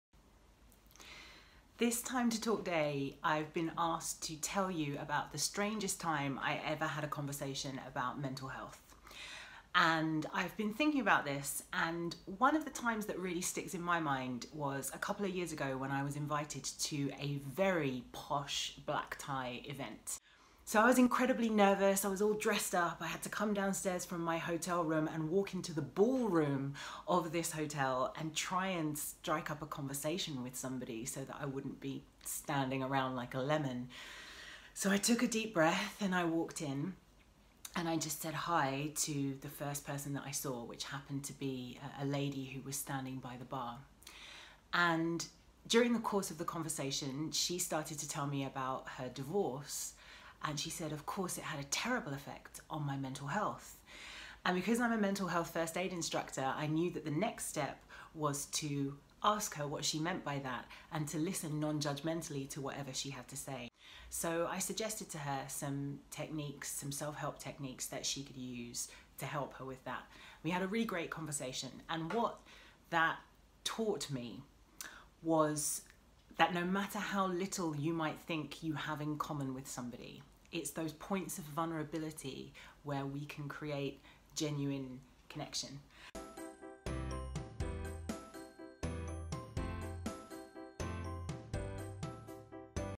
Time To Talk Day - Natasha Devon
For Time To Talk Day, we've been talking about all the places  our MHFAiders and instructors are starting conversations about mental health - from the rugby pitch to the barbershop. But sometimes the most important conversations happen when you least expect them.

Mental Health First Aid instructor Natasha Devon MBE spoke to us about her experience of using her MHFA skills when she found herself having a conversation about mental health somewhere she really didn't expect - and how it had a surprising impact for all involved.